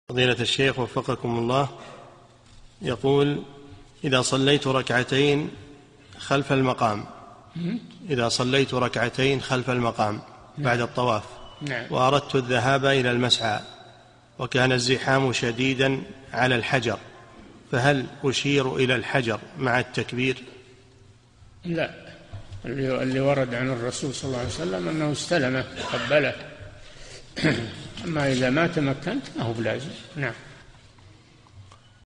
[1363 -3022] إذا كان الزحام شديداً على الحَجر، فهل أشير إلى الحجر مع التكبير؟ - الشيخ صالح الفوزان
-"[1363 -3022] السؤال: إذا صليت ركعتين خلف المقام بعد الطواف، وأردت الذهاب إلى المسعى، وكان الزحام شديداً على الحَجر، فهل أشير إلى الحجر مع التكبير؟
الجَوَابُ: لا، الذي ورد عن الرسول صلى الله عليه وسلم أنه استلمه وقبله، أَمَّا إذا ما تمكّنت ما هو بلازم، نعم.
للاشتراك في قناة الشيخ صالح الفوزان

للاستفسار والإقتراحات والدعم
رئيس الفريق
لمشروع كبار العلماء